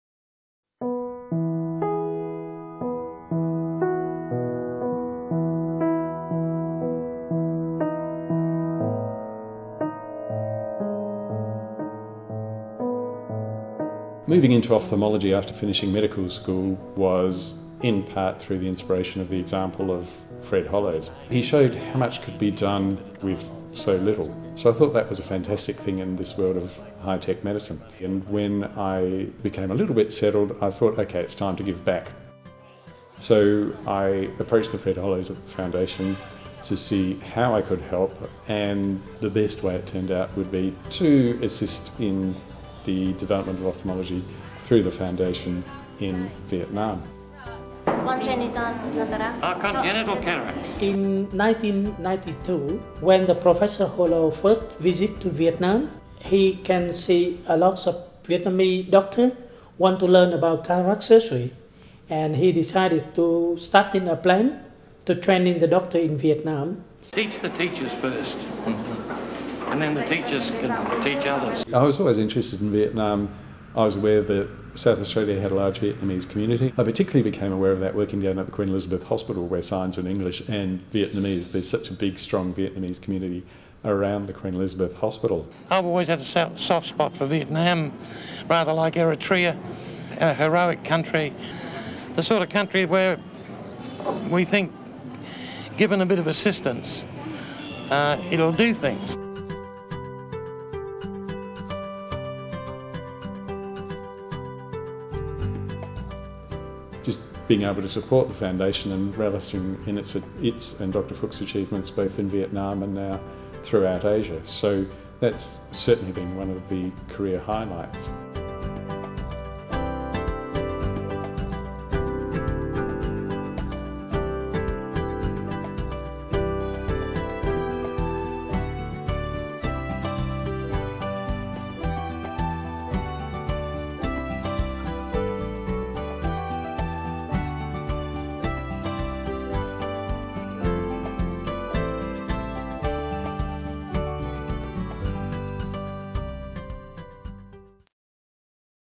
Dr Graham Fraenkel working with The Fred Hollows Foundation
Laser Vision SA's Dr Graham Fraenkel talks about his experience working with the fantastic Fred Hollows Foundation and her incredible work in Vietnam.